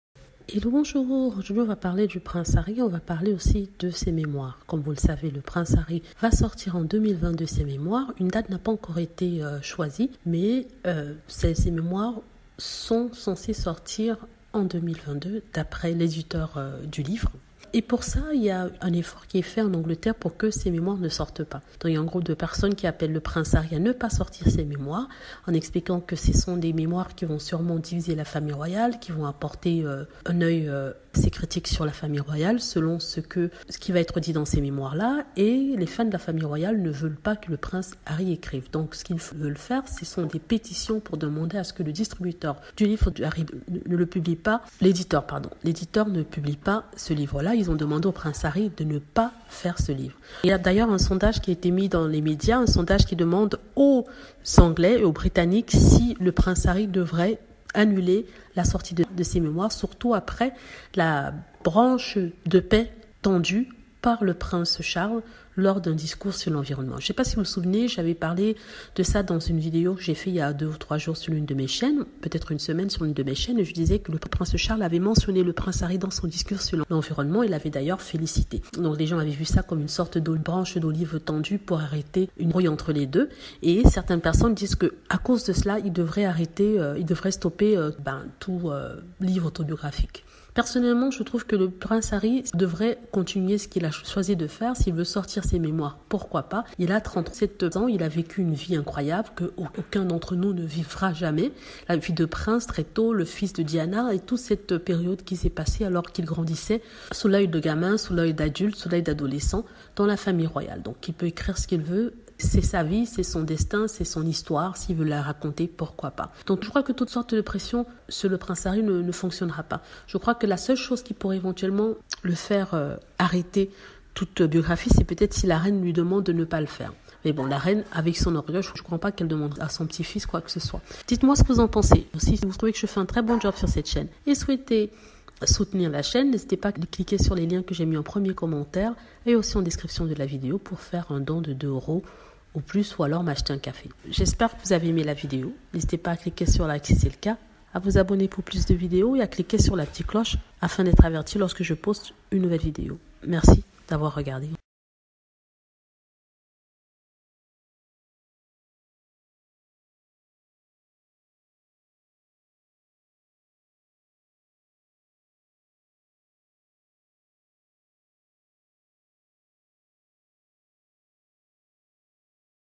les mémoires du Prince Harry le Duc de Sussex de nouveau critiqués. qui veut empêcher sa sortie?
Le livre de Harry le  Duc de Sussex devrait sortir en 2022. c'est en tout cas ce qu'a avancé l’éditeur de ce dernier. or tout est mis en œuvre pour que ce livre ne sorte pas. on avance qu'il sera très critique envers la famille royale et que le mari de meghan markle risque de révéler des secrets. une pétition a même été lancée pour que la maison d’édition du papa de archie ne publie pas son livre.

Si vous souhaitez soutenir la chaîne merci de donner une ou deux pièces, en utilisant l'un des liens ci-dessous merci d'avance.